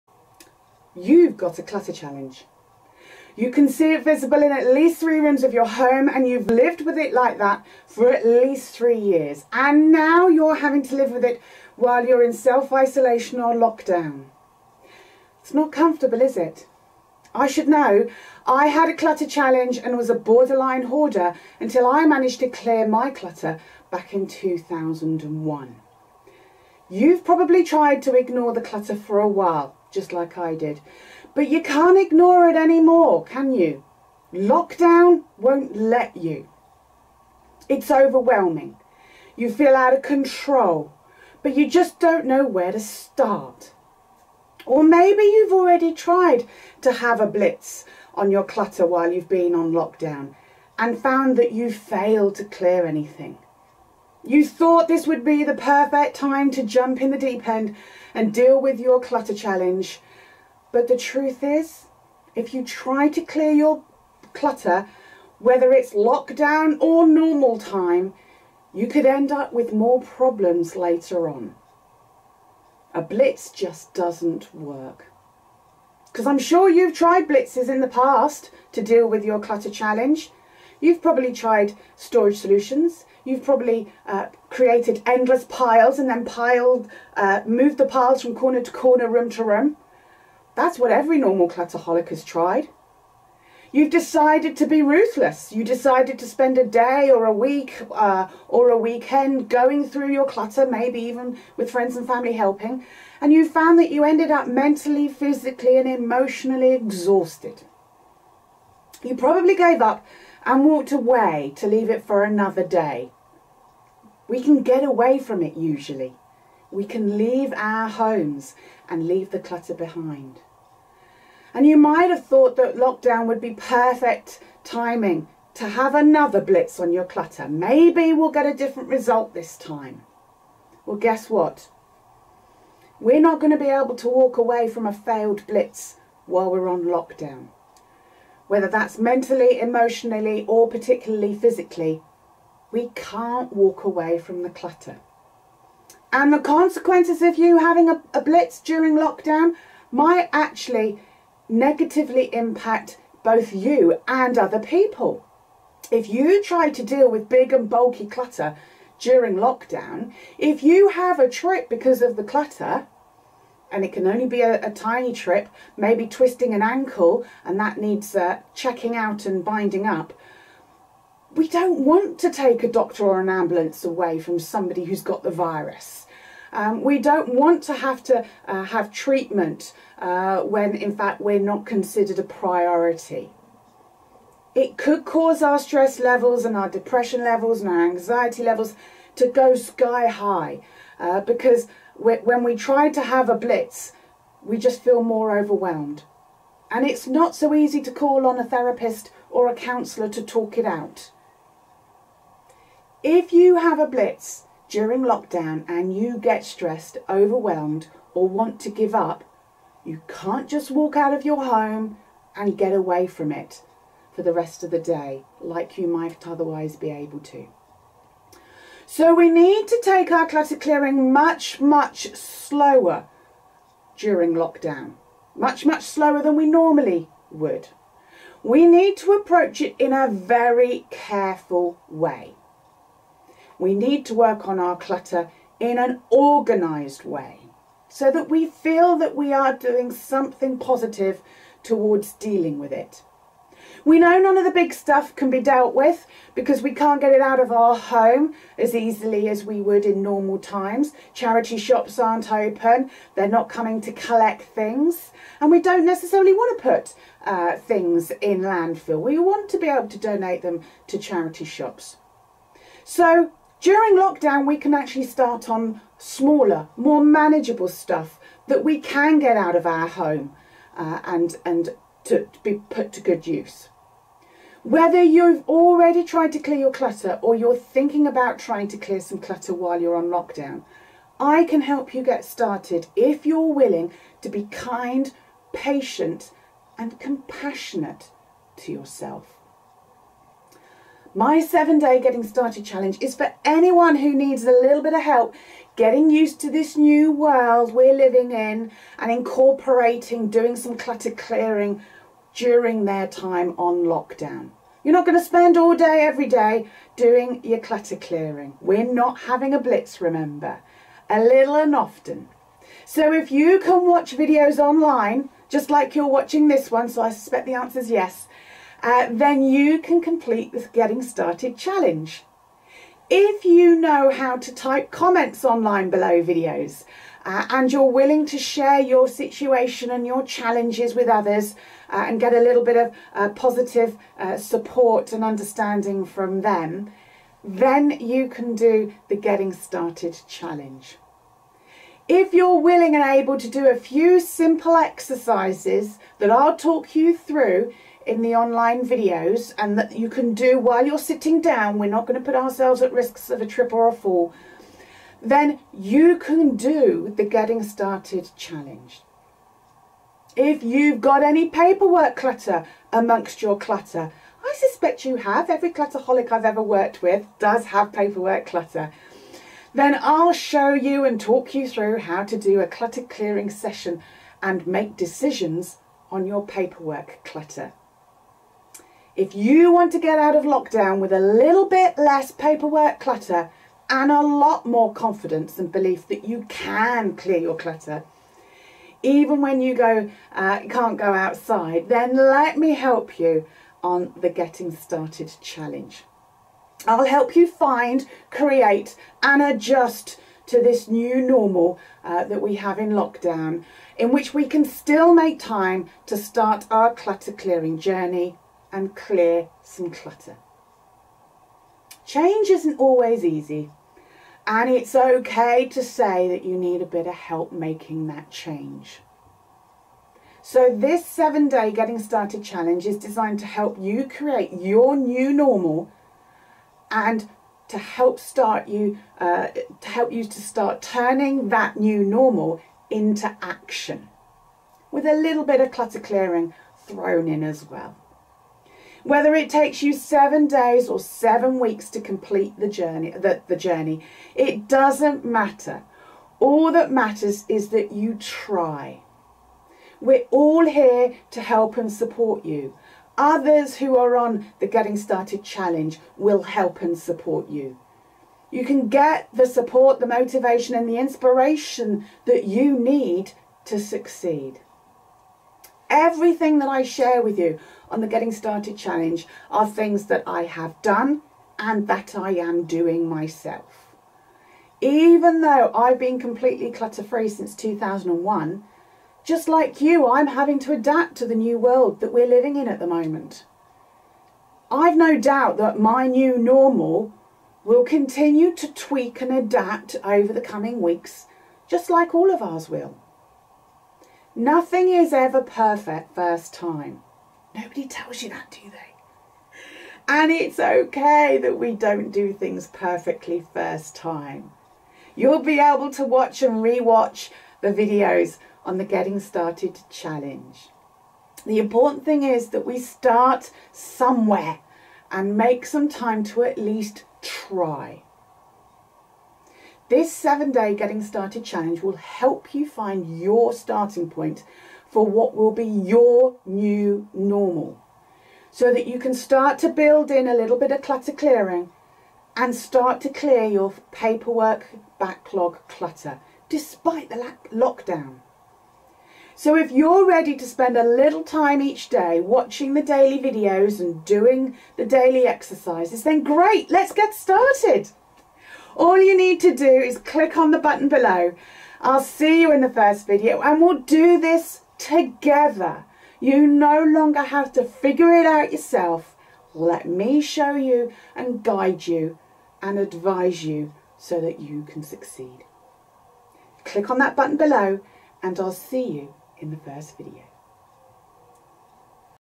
Getting Started
We've all been on lockdown for a while now, and hopefully you've managed to start to settle into a 'new normal' and some form of routine.

If you are Locked down with your clutter you may be struggling to adapt, or maybe you're trying to do something positive towards your Clutter Clearing.

If it's a bit overwhelming, you need a bit of help getting started and you'd like some support then I just wanted to know that my online 7 Day Getting Started Challenge video course is now available.

Whether it takes you 7 days or 7 weeks to complete it doesn't matter - the feedback, learning, and support from others that you will get will help motivate and inspire you as you continue your Clutter Clearing during Lockdown.